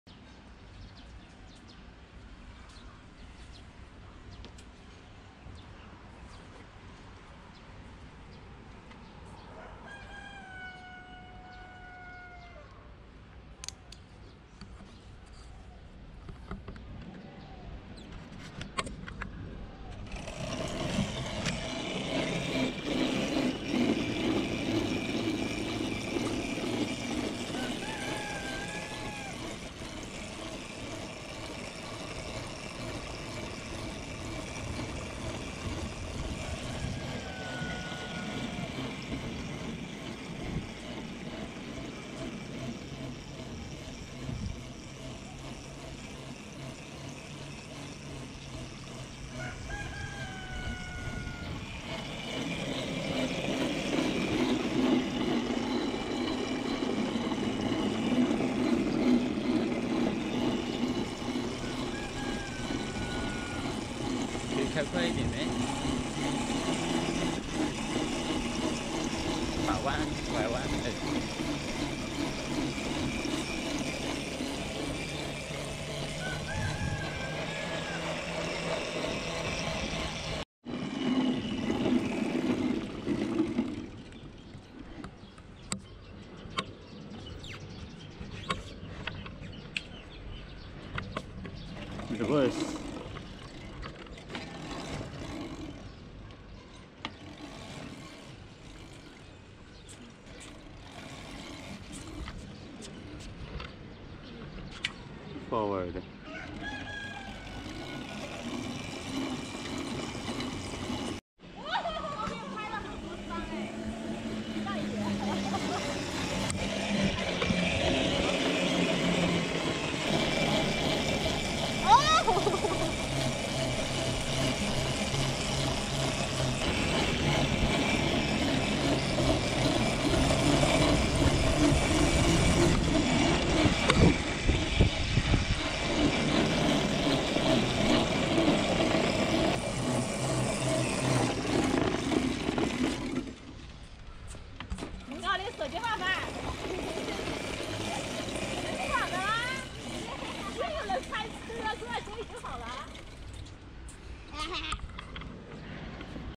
4K New Kids Electric Go Kart Practical Testing in Basketball Ground
The new electic go kart is running very smoothly in basketball playground. The go kart is design for kids, but with extra adult in the kart, it is running still smoothly and fast.